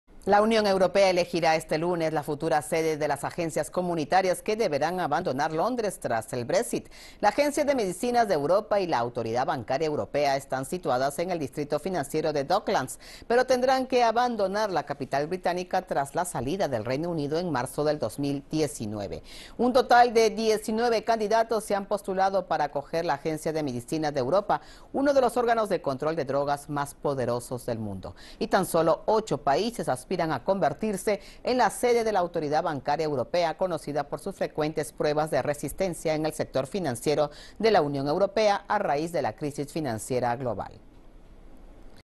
La UE elegirá las futuras sedes de las agencias comunitarias con sede en Londres
La Unión Europea elegirá este lunes las futuras sedes de las agencias comunitarias que deberán abandonar Londres tras el Brexit. La Agencia de Medicinas de Europa y la Autoridad Bancaria Europea están situadas en el distrito financiero de Docklands, pero tendrán que abandonar la capital británica tras la salida de Reino Unido en marzo de 2019.